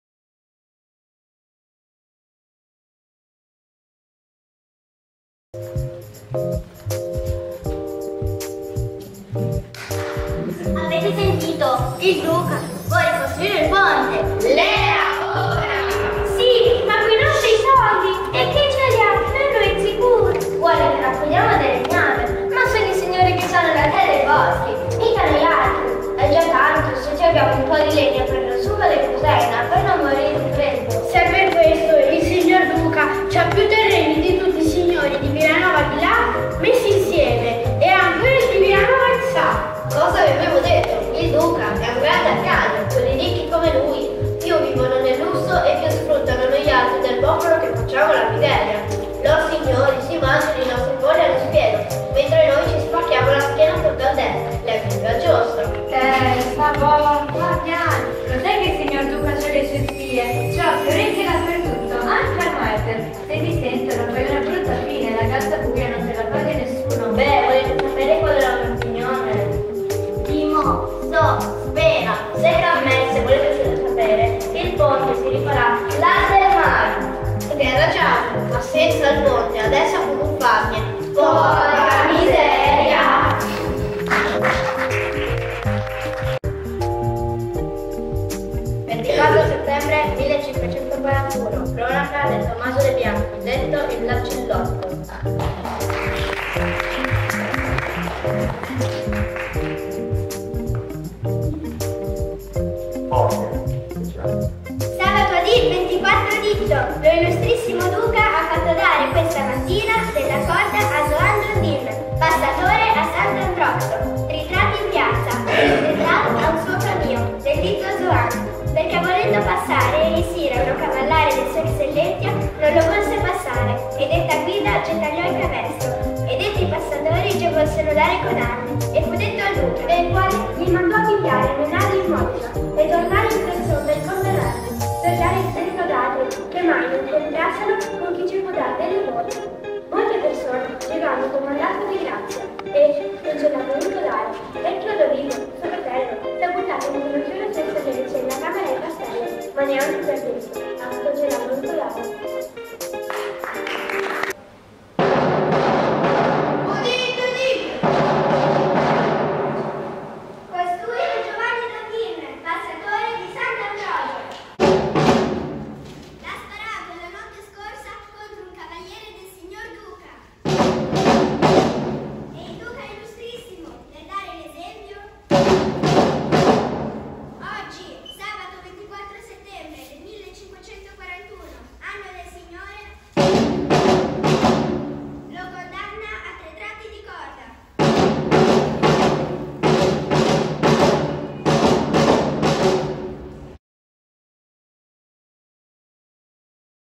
La storia va in scena
Dopo la visita in Archivio sulla documentazione storica, i bambini e le bambine delle classi quinte della scuola primaria Ciro Menotti di Villanova, hanno messo in scena storie e protagonisti del passato della loro località, ridando vita alla figura del barcaiolo (passatore) immaginando un suo incontro con il Duca estense e i suoi funzionari.

A cura di Archimedia s.r.l.